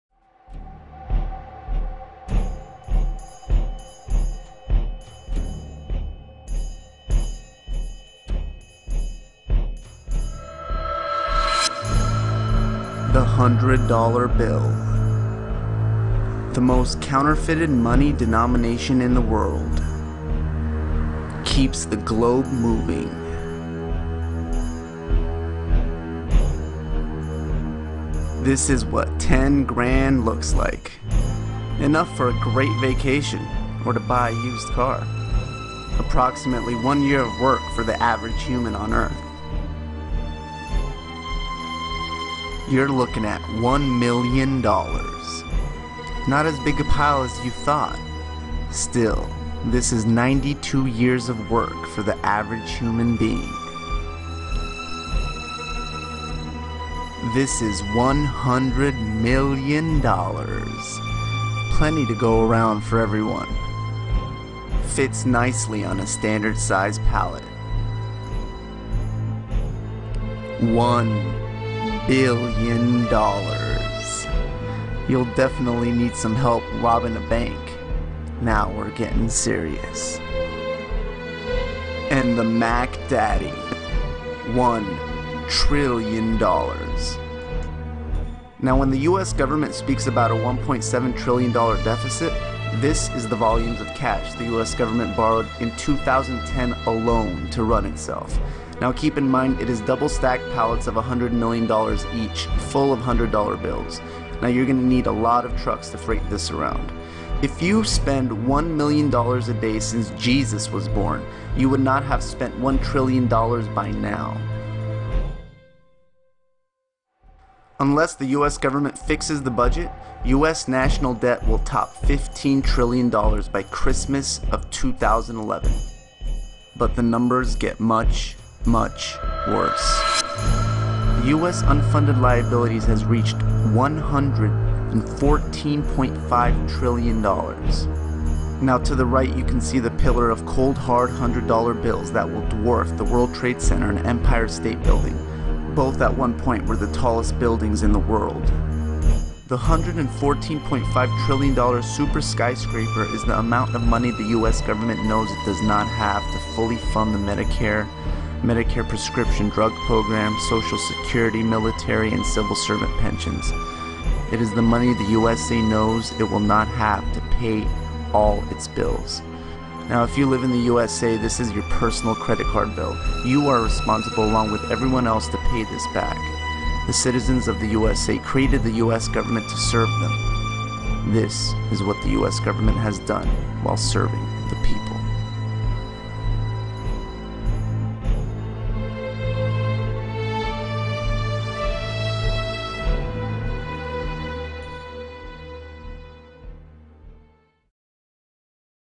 $16 Trillion U.S. DEBT - A Visual Perspective SCARY AS HELL !!!!
This is a very real and scary perspective that is going on right now and we must do everything we can to stop it.  ASAP